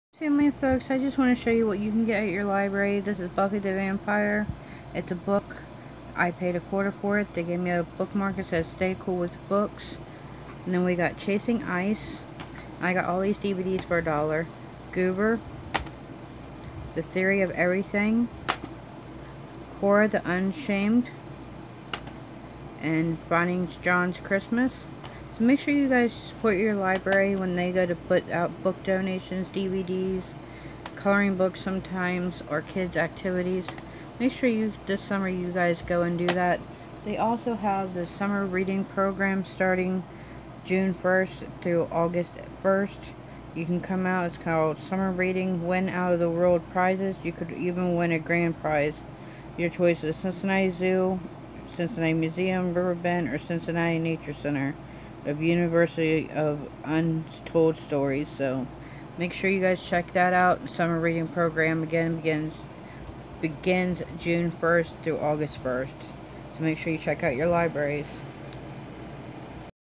library trip from Wednesday night
summer reading starts 06/01/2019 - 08/01/2019
make sure to sign up at your local library
I sign up at williamsburg local library